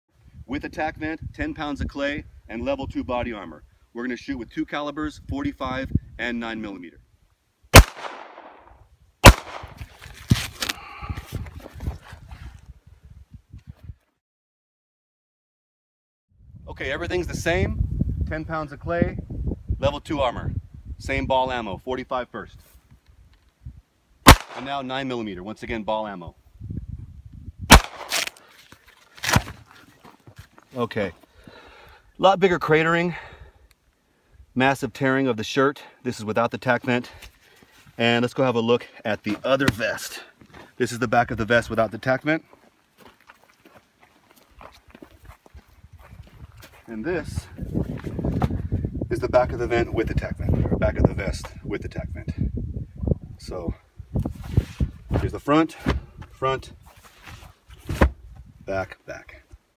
TacVent Ballistic Test
How does the 1/2" gap affect ballistics of body armor? Have a look. Though the intended purpose is the TacVent is heat ventilation, profound trauma attenuation turns out to be a huge bonus.

The cop-invented TacVent is a lightweight, highly flexible, customizable rubbery plastic insert comprised of corrugated, vented grooves, designed to be placed between ballistic armor and the user's body, providing 1/2" of comfortable separation and allowing constant, passive and significant ventilation of heat, over 15°, while minimizing moisture and odor, something that has never been done before. A huge bonus is that the TacVent profoundly minimizes blunt force trauma!